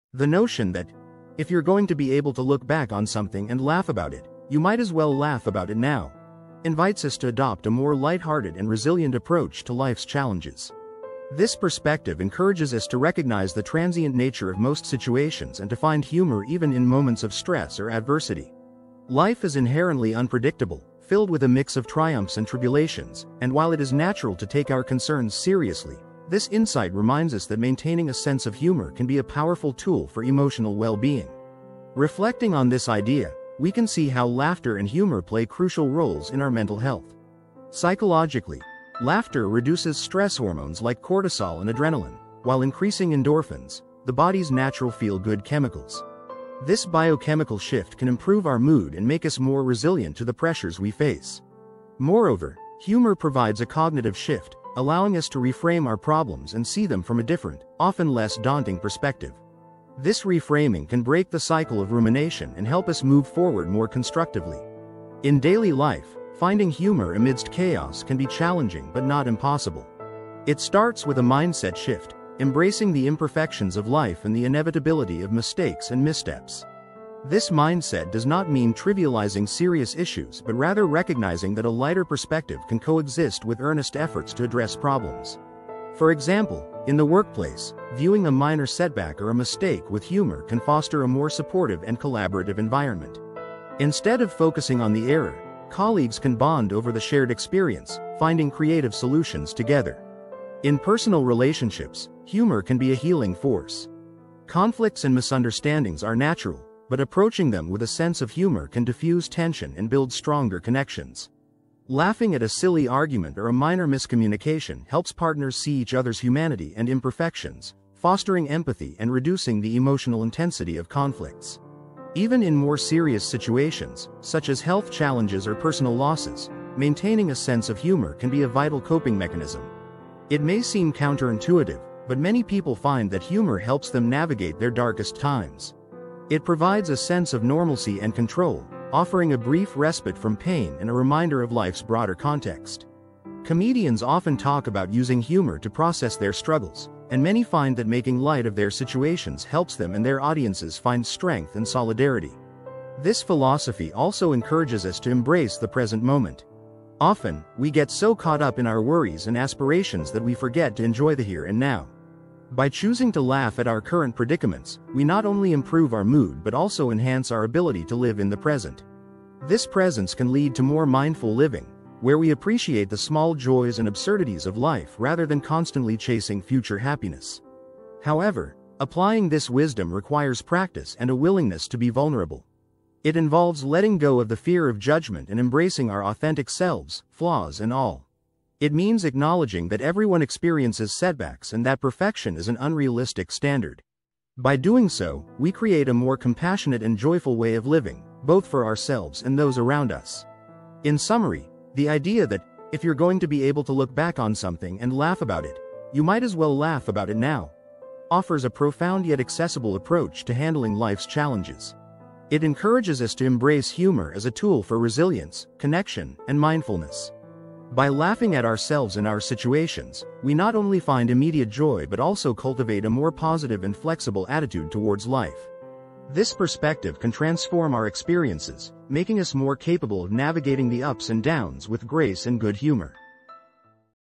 Why Laughing at Your Problems Now Can Change Your Life!
The notion that "If you're going to be able to look back on something and laugh about it, you might as well laugh about it now" invites us to adopt a more light-hearted and resilient approach to life's challenges. This perspective encourages us to recognize the transient nature of most situations and to find humor even in moments of stress or adversity. Life is inherently unpredictable, filled with a mix of triumphs and tribulations, and while it is natural to take our concerns seriously, this insight reminds us that maintaining a sense of humor can be a powerful tool for emotional well-being.

Reflecting on this idea, we can see how laughter and humor play crucial roles in our mental health. Psychologically, laughter reduces stress hormones like cortisol and adrenaline, while increasing endorphins, the body's natural feel-good chemicals. This biochemical shift can improve our mood and make us more resilient to the pressures we face. Moreover, humor provides a cognitive shift, allowing us to reframe our problems and see them from a different, often less daunting perspective. This reframing can break the cycle of rumination and help us move forward more constructively.

In daily life, finding humor amidst chaos can be challenging but not impossible. It starts with a mindset shift, embracing the imperfections of life and the inevitability of mistakes and missteps. This mindset does not mean trivializing serious issues but rather recognizing that a lighter perspective can coexist with earnest efforts to address problems. For example, in the workplace, viewing a minor setback or a mistake with humor can foster a more supportive and collaborative environment. Instead of focusing on the error, colleagues can bond over the shared experience, finding creative solutions together.

In personal relationships, humor can be a healing force. Conflicts and misunderstandings are natural, but approaching them with a sense of humor can defuse tension and build stronger connections. Laughing at a silly argument or a minor miscommunication helps partners see each other's humanity and imperfections, fostering empathy and reducing the emotional intensity of conflicts.

Even in more serious situations, such as health challenges or personal losses, maintaining a sense of humor can be a vital coping mechanism. It may seem counterintuitive, but many people find that humor helps them navigate their darkest times. It provides a sense of normalcy and control, offering a brief respite from pain and a reminder of life's broader context. Comedians often talk about using humor to process their struggles, and many find that making light of their situations helps them and their audiences find strength and solidarity.

This philosophy also encourages us to embrace the present moment. Often, we get so caught up in our worries and aspirations that we forget to enjoy the here and now. By choosing to laugh at our current predicaments, we not only improve our mood but also enhance our ability to live in the present. This presence can lead to more mindful living, where we appreciate the small joys and absurdities of life rather than constantly chasing future happiness.

However, applying this wisdom requires practice and a willingness to be vulnerable. It involves letting go of the fear of judgment and embracing our authentic selves, flaws and all. It means acknowledging that everyone experiences setbacks and that perfection is an unrealistic standard. By doing so, we create a more compassionate and joyful way of living, both for ourselves and those around us.

In summary, the idea that "If you're going to be able to look back on something and laugh about it, you might as well laugh about it now" offers a profound yet accessible approach to handling life's challenges. It encourages us to embrace humor as a tool for resilience, connection, and mindfulness. By laughing at ourselves and our situations, we not only find immediate joy but also cultivate a more positive and flexible attitude towards life. This perspective can transform our experiences, making us more capable of navigating the ups and downs with grace and good humor.